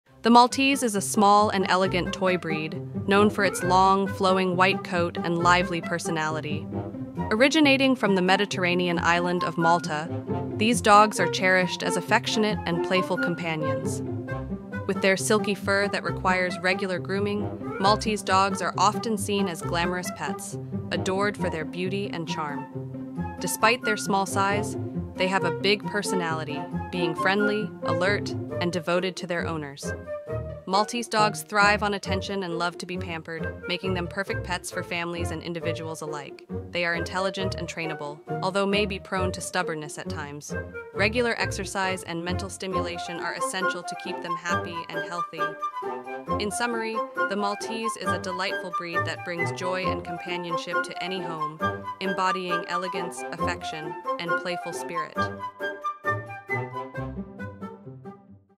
Maltese: The Elegant Toy Companion
Explore the world of the Maltese, a captivating toy breed celebrated for its beauty and affectionate nature. Originating from the Mediterranean, these small dogs boast a luxurious white coat and a lively personality that steals hearts wherever they go. In this video, delve into the charm and elegance of the Maltese breed, discovering their history, care needs, and the joy they bring to families. Whether you're a seasoned Maltese owner or considering adding one to your family, this guide offers valuable insights into the world of these delightful companions